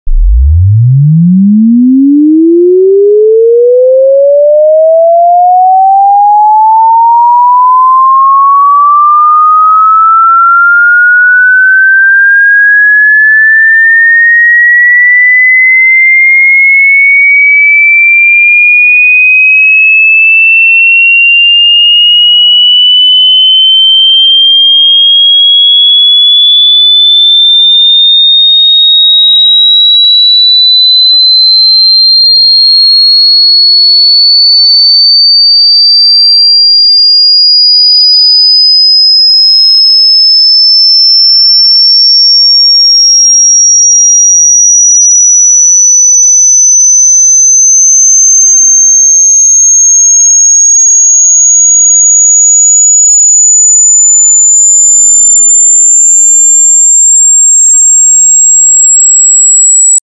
20 - 7800 Hz Sweep Tone Audio Sweep Ton wechselnde Frequenz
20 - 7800 Hz Sweep Sinus Testton
0 DB Stereo
60 Sekunden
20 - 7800  Hz sweep sine test tone
0 DB Stereo
60 sec.